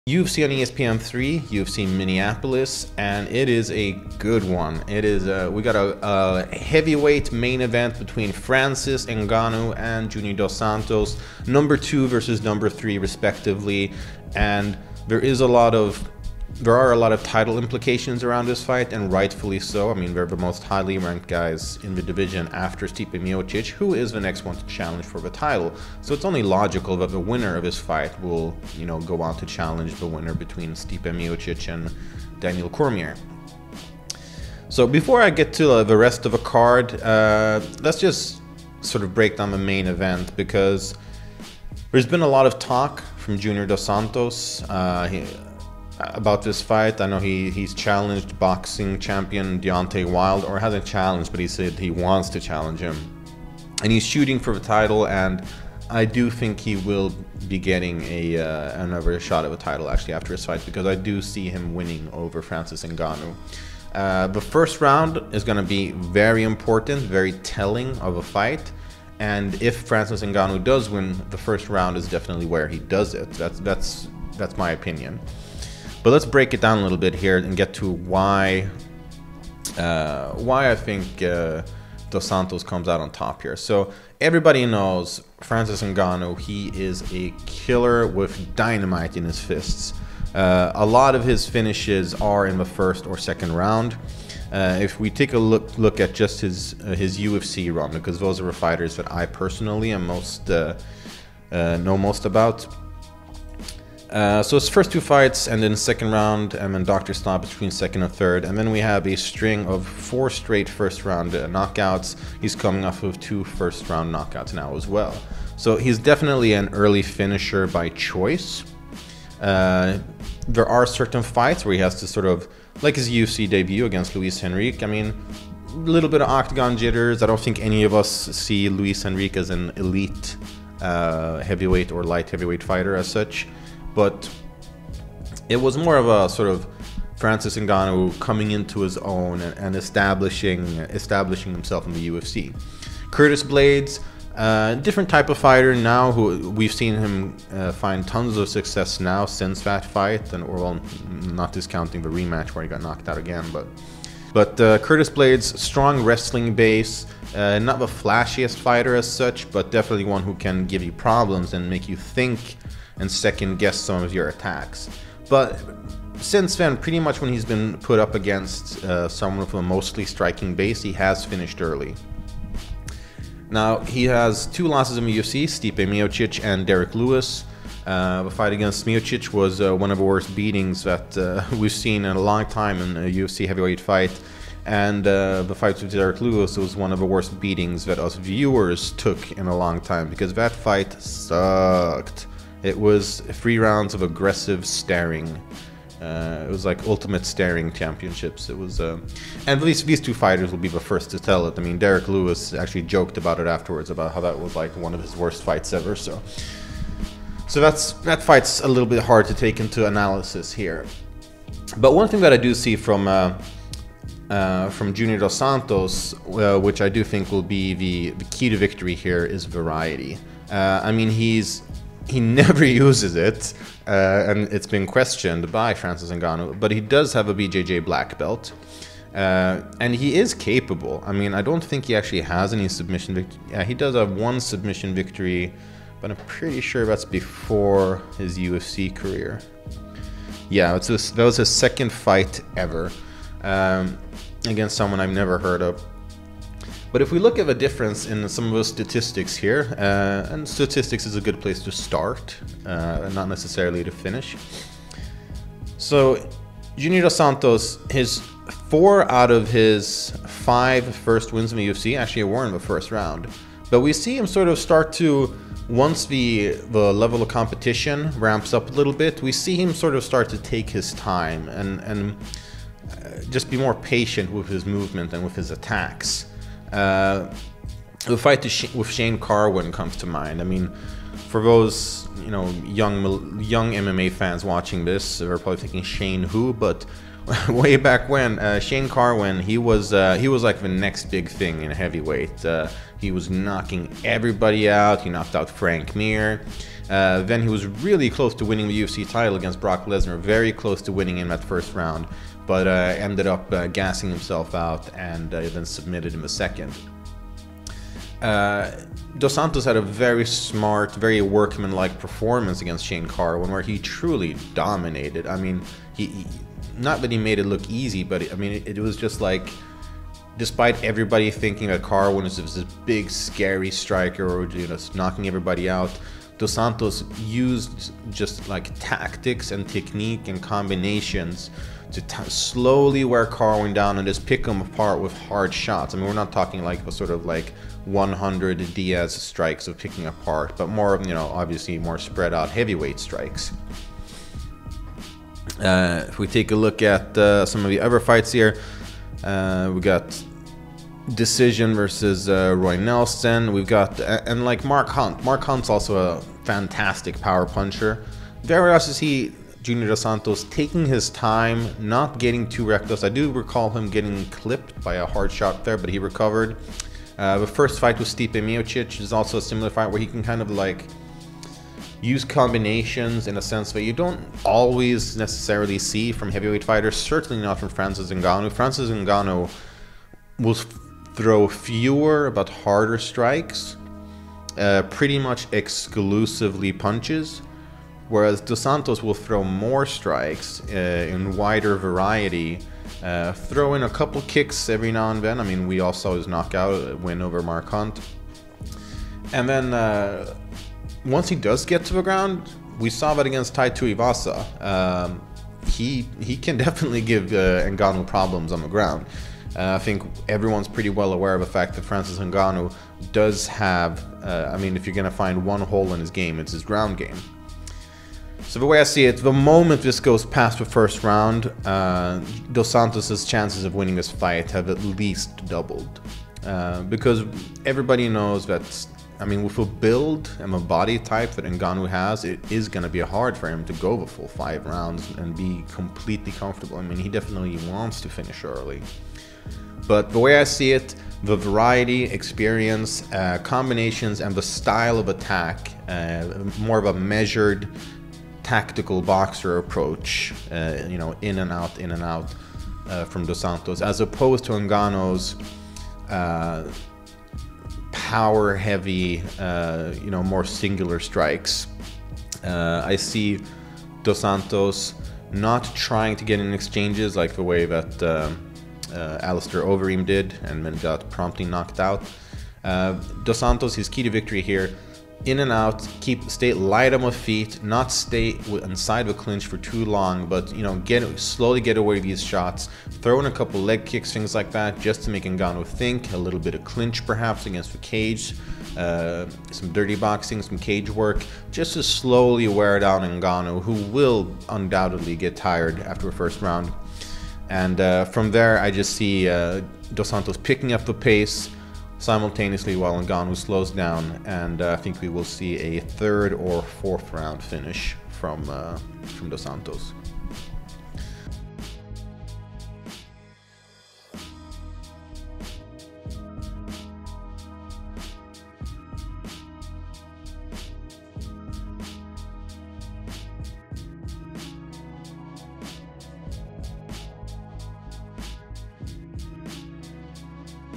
Real Talk: Why Junior dos Santos wins over Francis Ngannou
In this clip from MMAnytt Real Talk we break down the heavyweight main event at UFC On ESPN 3 between Junior dos Santos and Francis Ngannou.